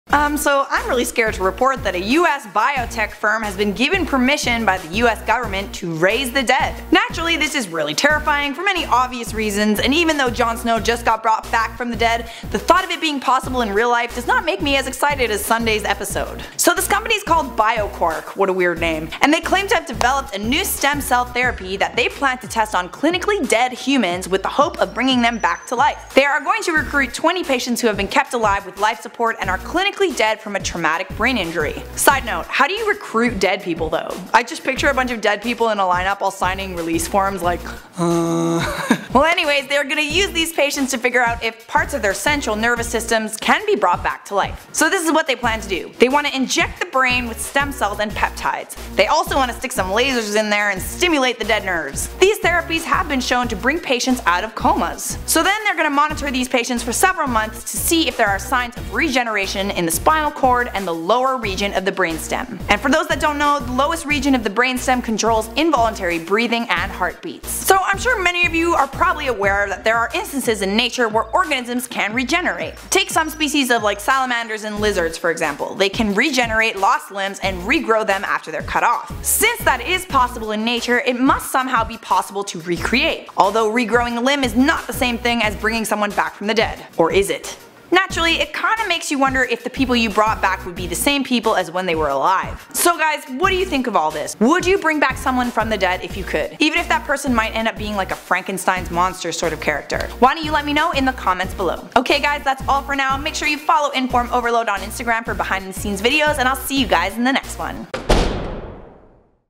Biotech Firm Was Just Given Permission To Create Zombies
U.S. government gave permission to Bioquark, to test a new stem cell therapy on humans who are clinically dead, in the hopes that they can bring them back to life.

2408 Lakeshore Blvd W.